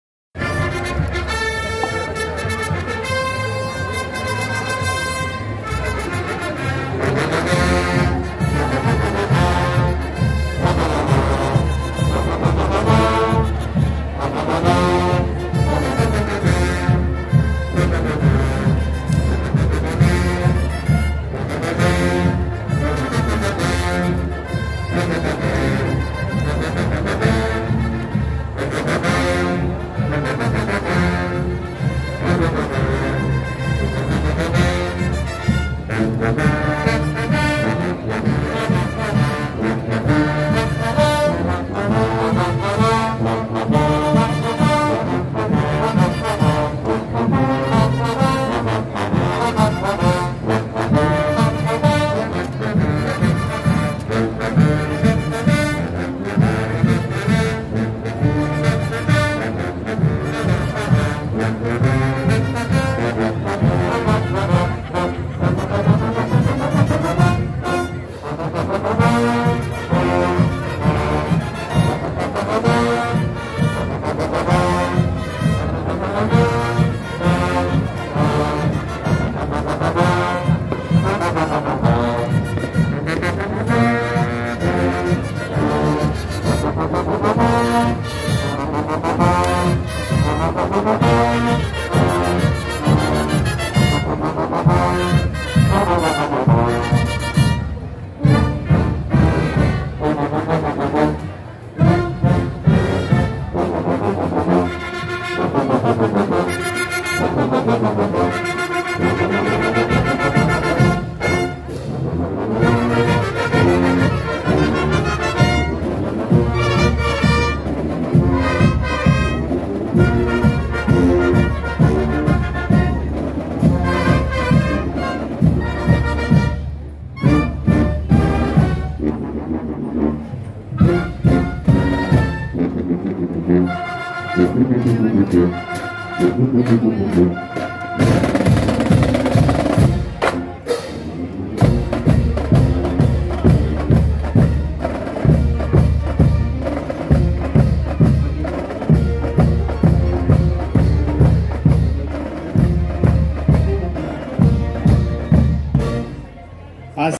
Colegio Ingeniero Tomás Guardia, Desfile 12 de septiembre del 2023, Parte 5
Desfile 12 de septiembre del 2023 en La Chorrera.
Recomiendo siempre ver los videos de las bandas musicales con audífonos para una mejor experiencia.